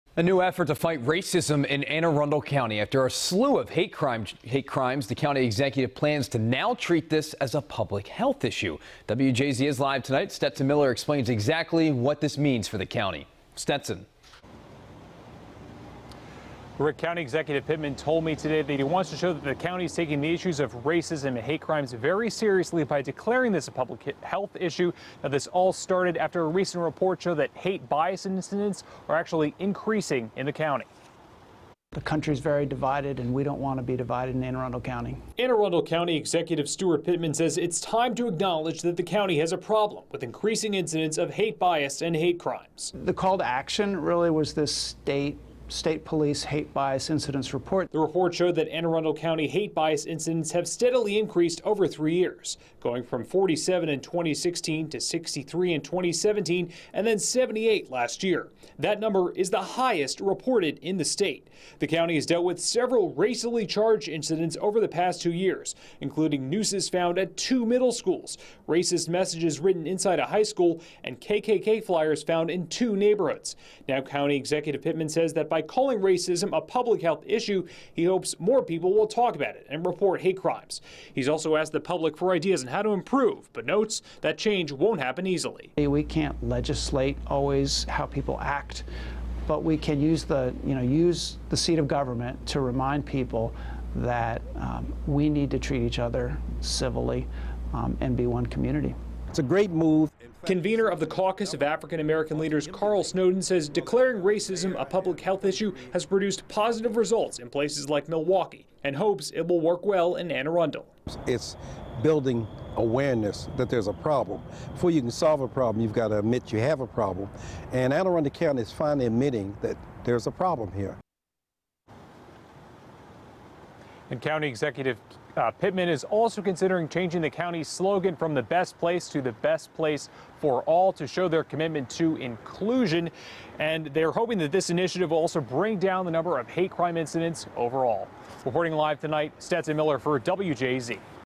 County Executive Steuart Pittman To Treat Racism As Public Health Issue In AACO
Anne Arundel County Executive Steuart Pittman is planning to treat racism as a public health issue.